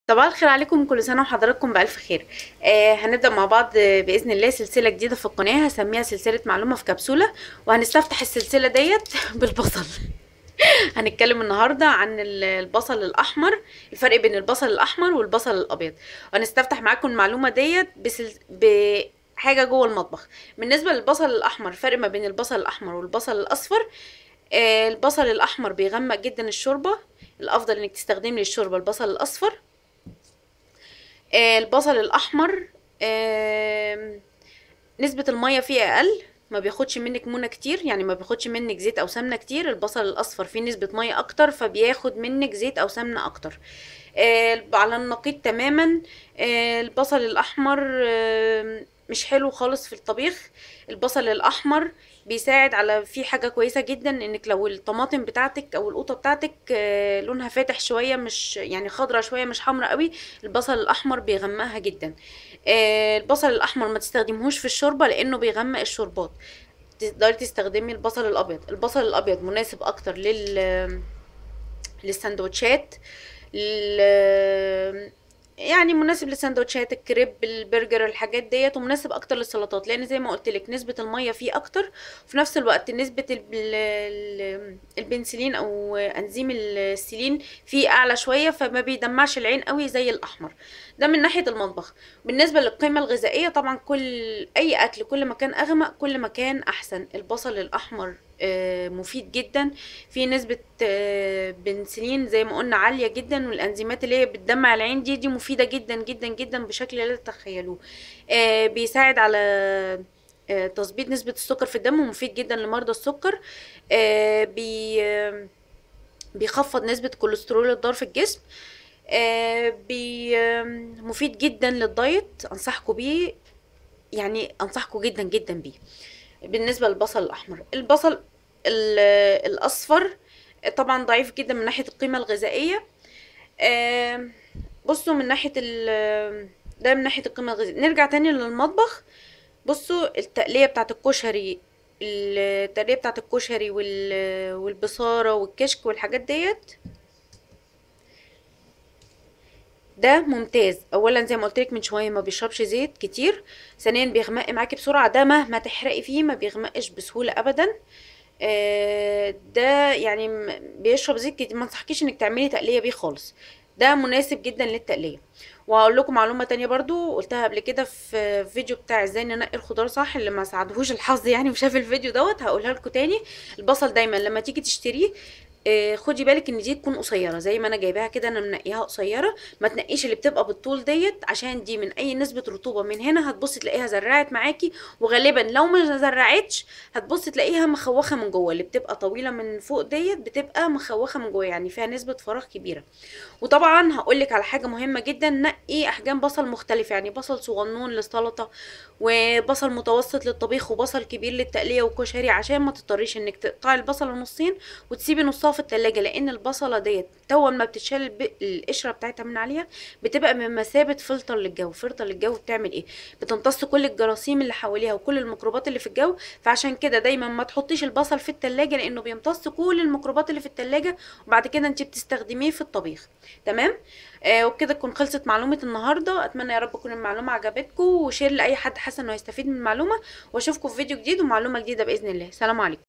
الفرق بين البصل الاحمر ..والبصل الاصفر (سلسلة معلومة ف كبسولة ..ح1)
استخدامات البصل لا تعد ولا تحصي..سواء للمطبخ او استخدام البصل للتجميل او حتي
استخدام البصل كا علاج بديل لبعض الامراض..وللمحافظة على  رشاقة  الجسم..في هذا الفيديو
سنستعرض سويا..الفرق بين انواع البصل ..بصل اصفر او بصل احمر وفيما نستخدم كلا منهم